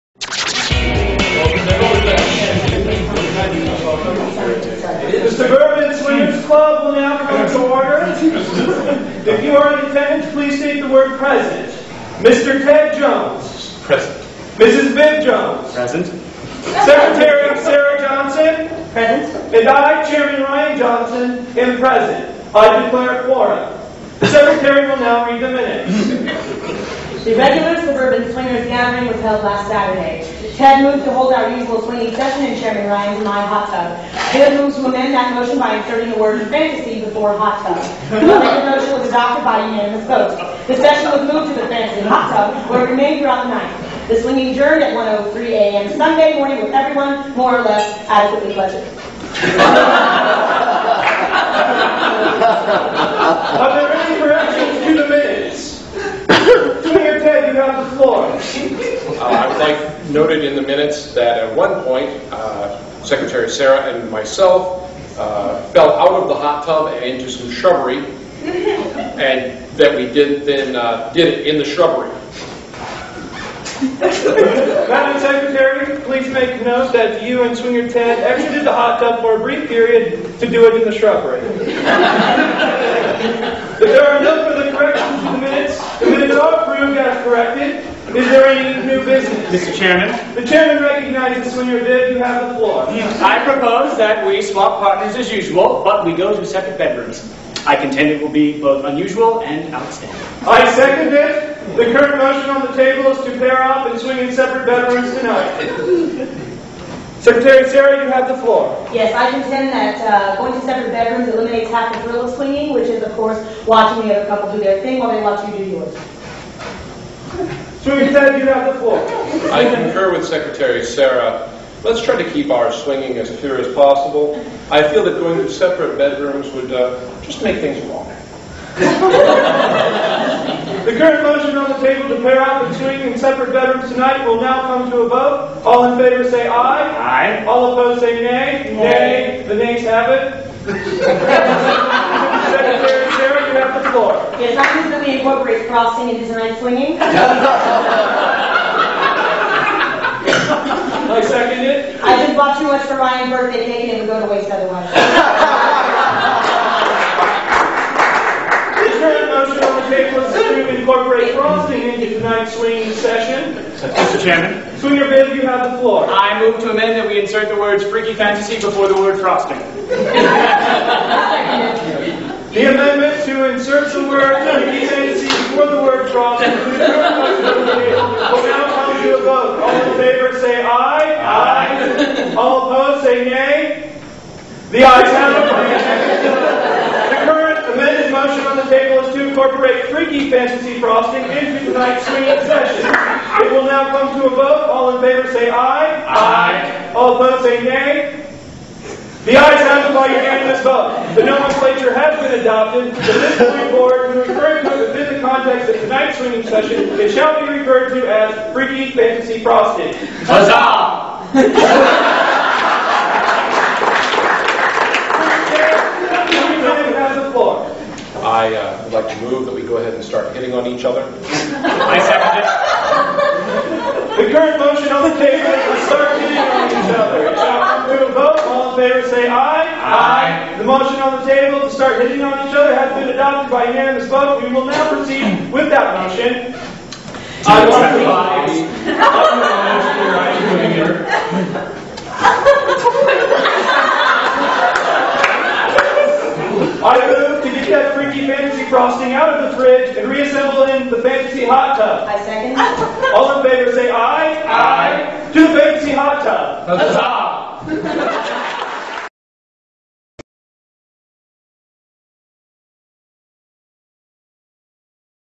Three Dollar Bill ComCo.!: Roberts Rules
A sketch called, "Roberts Rules," from Three Dollar Bill Comedy Company in Indianapolis, Indiana.  This was originally part of the revue, "No Child Laft Behind," in 2008.  Oh, Suburbia, is there nothing you suck the life out of?